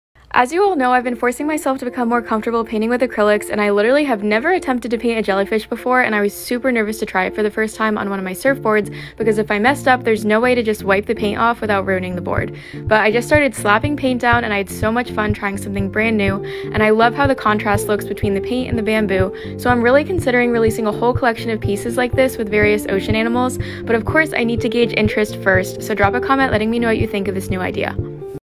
Bamboo surf board with ArtResin
We are loving the look of this jelly fish painted on a bamboo surf board!